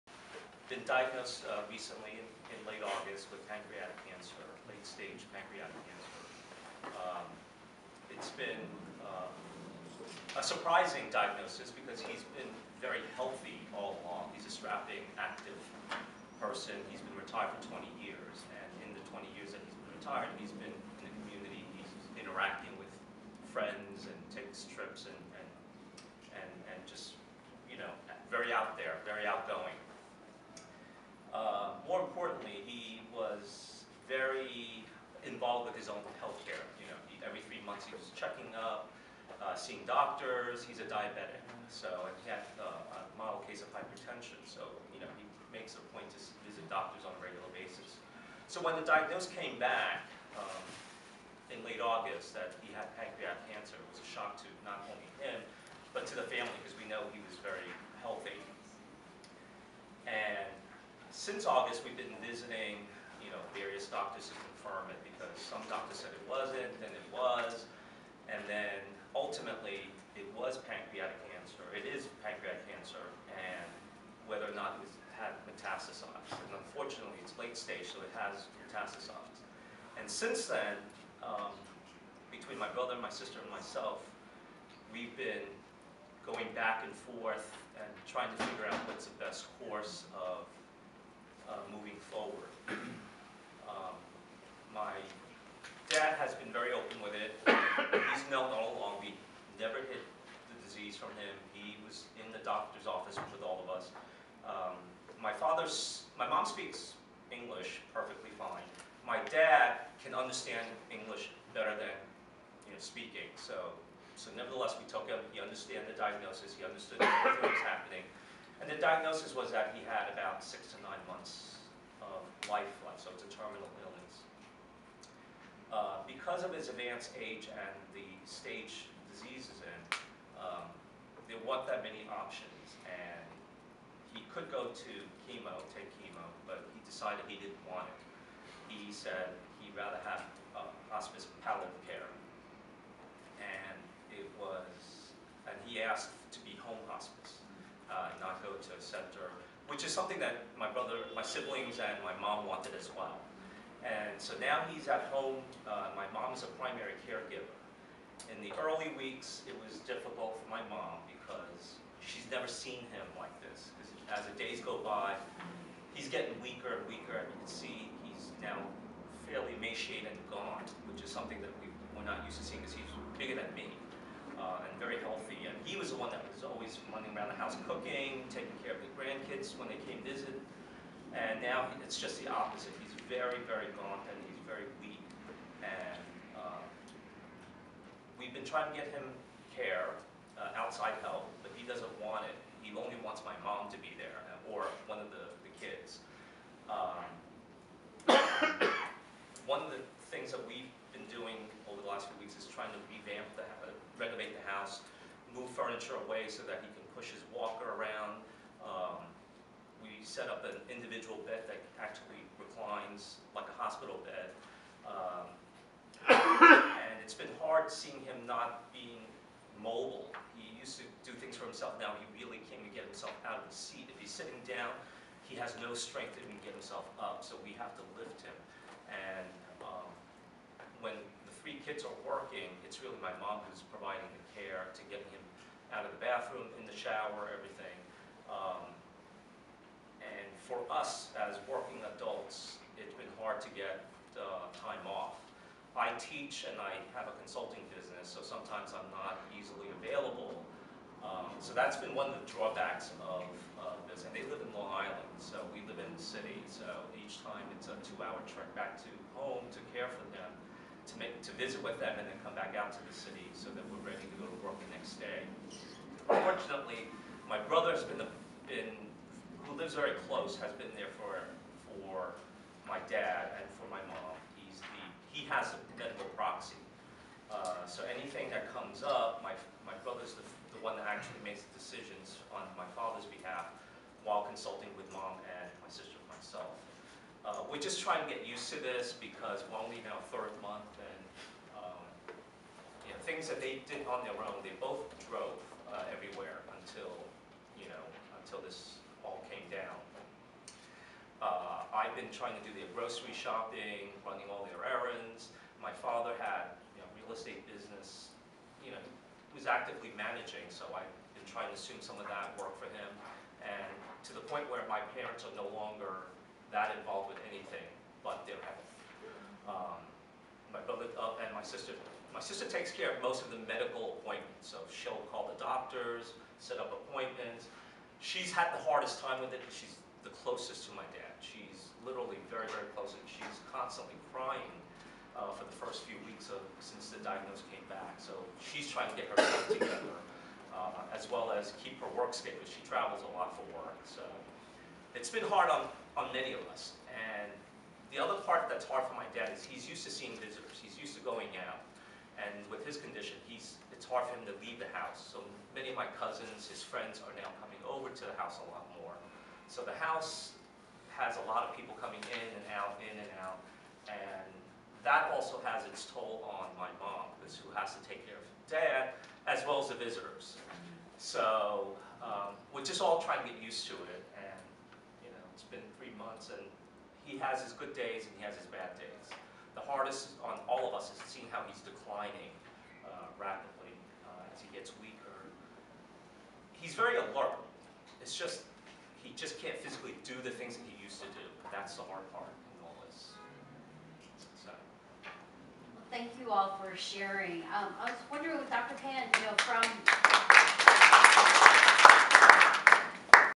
AAPI Caregiving Forum - David Fung
Asian American and Pacific Islander Caregiving Forum
Presented by: APIAVote, AARP and AARP NY

On November 21, 2017, APIAVote and AARP hosted a Caregiving Forum featuring special guest U.S. Congresswoman Grace Meng in celebration of November Caregiving Month. One quarter of caregivers are millennials! Every one of us will be a caregiver, and/or need care.  These videos will provide you the opportunity to hear and learn through others' experiences as they share needs and challenges of being a caregiver or being provided care.

SPEAKERS:
  Congresswoman Grace Meng
  Dr. Cynthia Pan, Chief, Division of Geriatrics and Palliative Care for
     NewYork-Presbyterian/Queens
  Myoung Mi Kim, Director of the Adult Day Care Program from the
     Korean Community Services.
  Glenn Magpantay, Executive Director, National Queer Asian Pacific
     Islander Alliance
  Dave Fung, Caregiver
  Chris Widelo, AARP New York
  Steven Raga, AARP National